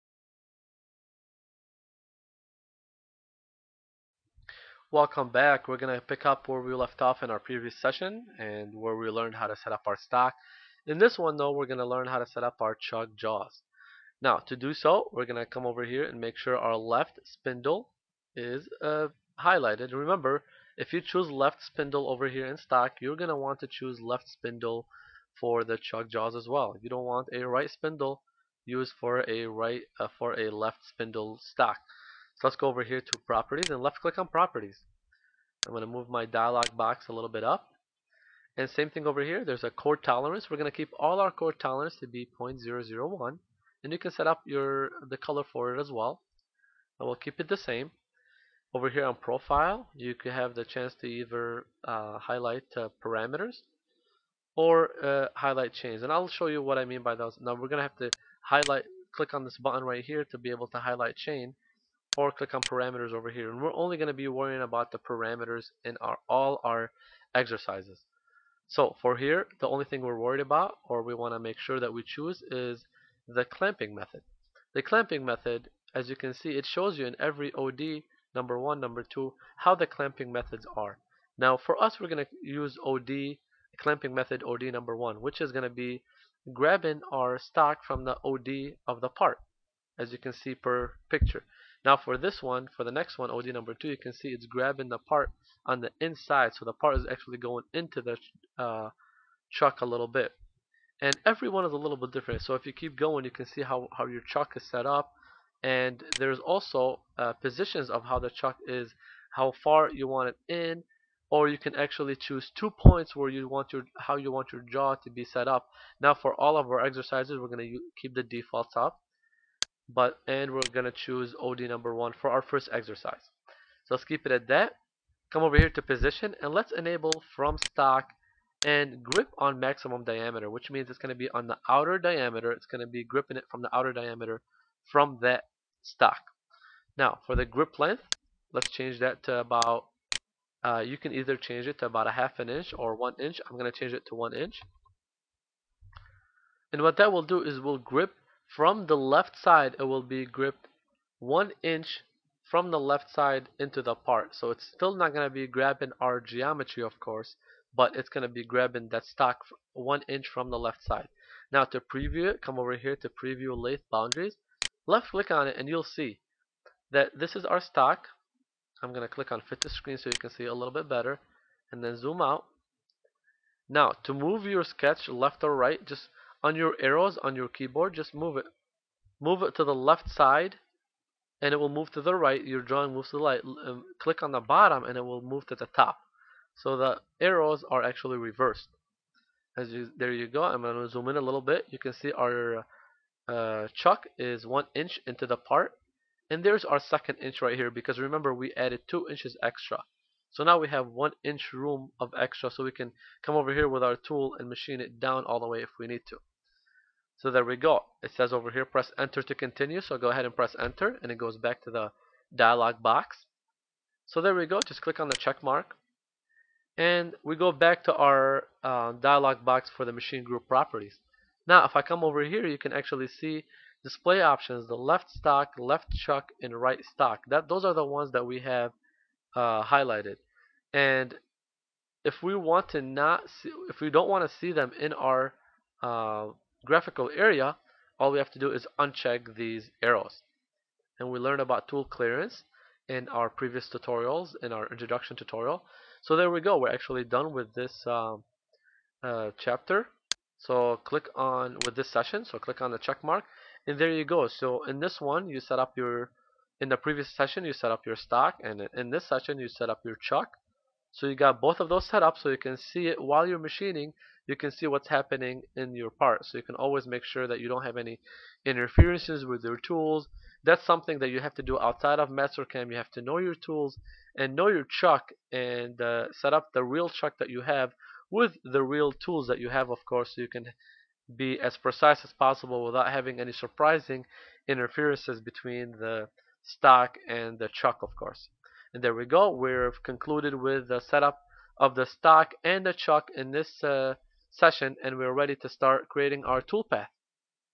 MASTERCAM X1-X7 LATHE Tutorial in HD - 1.4 Chuck Jaw Setup - vtpros.net
MASTERCAM X1-X6 LATHE EXERCISE 1 SESSION 4 - Learning Chuck Jaw Setup

FREE demo video tutorial from vtpros.net